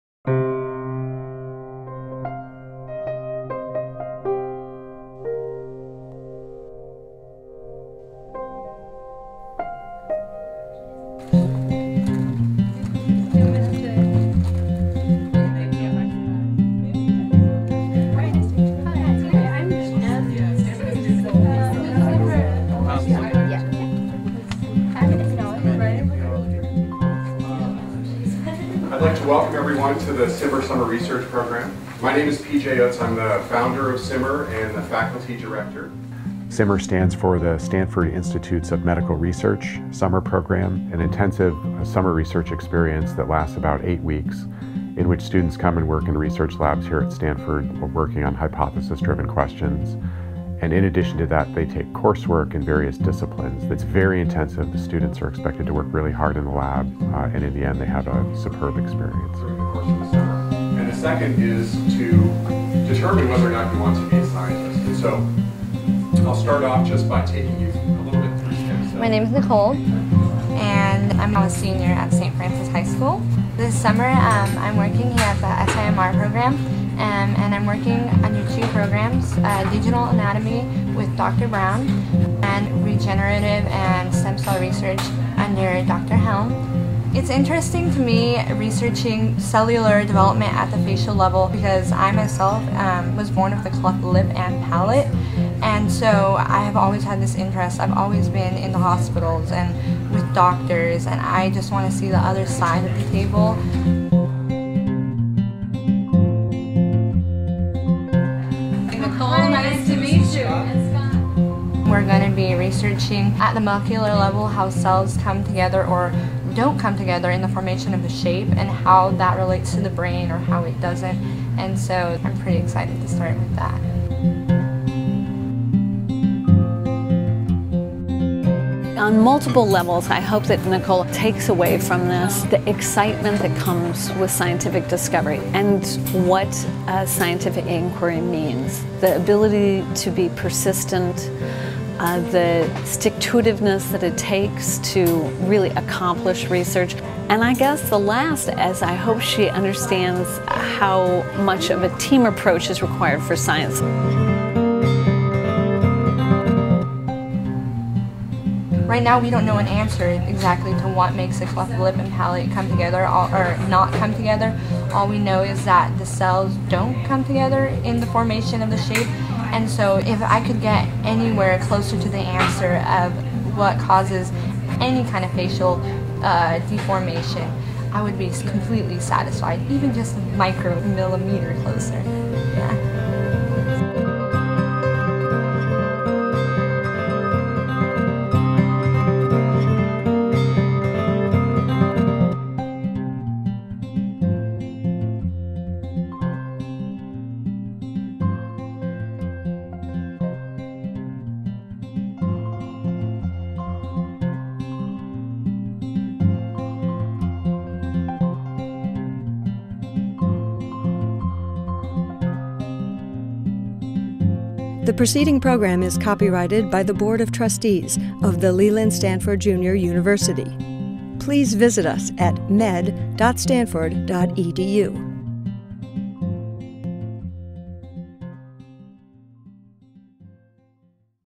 Know thyself: a summer intern tests the waters of scientific research
Every year a select group of high school students forego the lazy days of summer to get to work on cutting edge research projects at Stanford University School of Medicine's multiple internship programs. They follow their unique interests and passions and explore a variety of biomedical fields as they test the waters of a career in science. One intern in particular hopes that the experience will bring her a little closer to an understanding of herself.